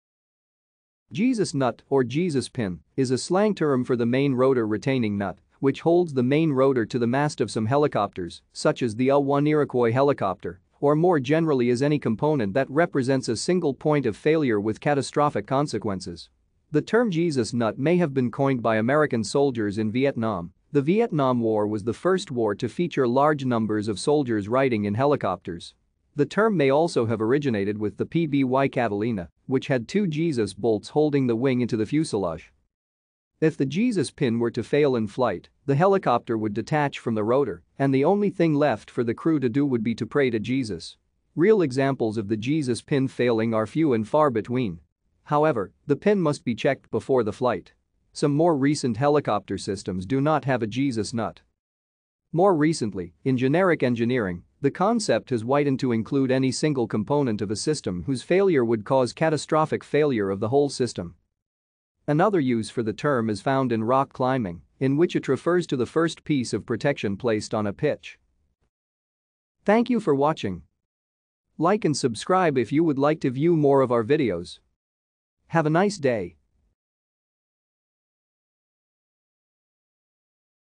Jesus Nut
Jesus nut, or Jesus pin, is a slang term for the main rotor retaining nut, which holds the main rotor to the mast of some helicopters, such as the UH-1 Iroquois helicopter; or more generally is any component that represents a single point of failure with catastrophic consequences. The term Jesus nut may have been coined by American soldiers in Vietnam; the Vietnam War was the first war to feature large numbers of soldiers riding in helicopters. The term may also have originated with the PBY Catalina, which had two Jesus bolts holding the wing into the fuselage.